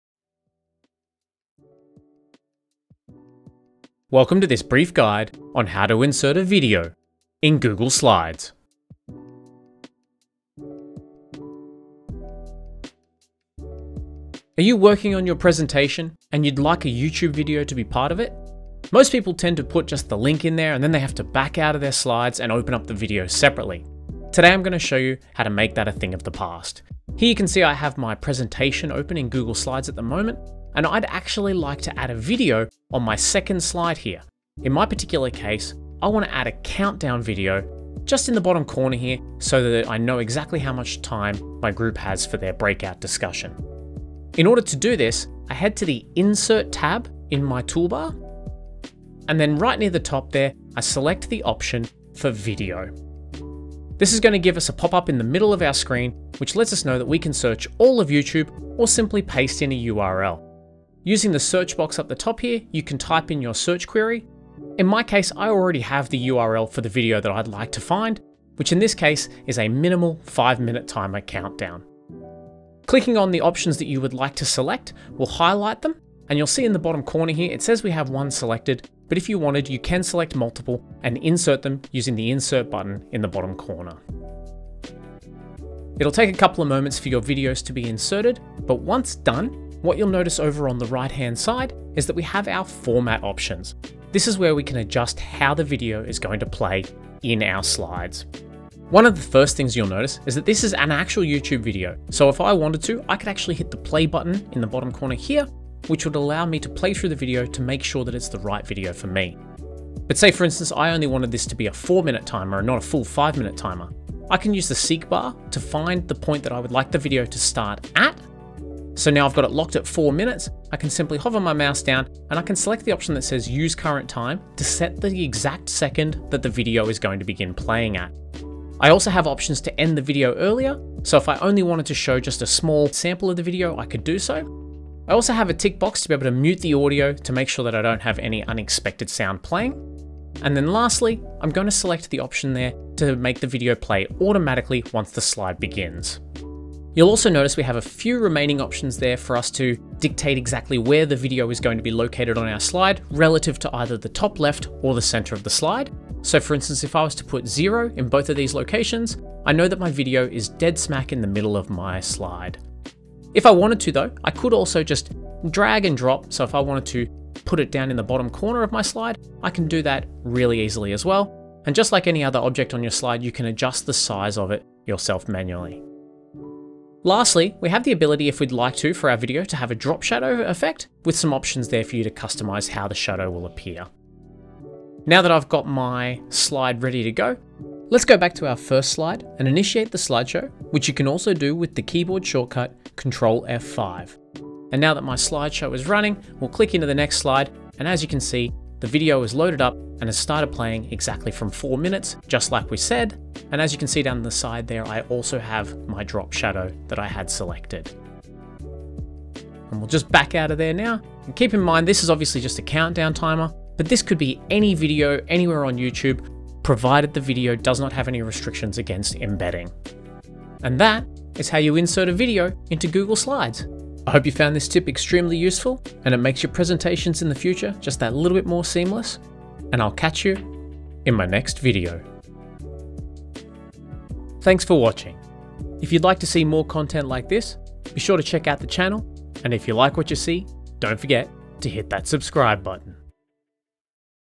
Embed YouTube videos in Google Slides!
In this video, I will show you how to elevate your presentation skills by adding videos to Google Slides. We will cover all the steps you need to take to make sure your presentations are engaging, professional, and informative.

You will learn how to add videos to Google Slides, how to adjust their size and position, and how to set them to play automatically from a set point to truly enhance your presentation content.
With this knowledge, you will be able to create presentations that stand out and captivate your audience.

Music from Uppbeat (free for Creators!)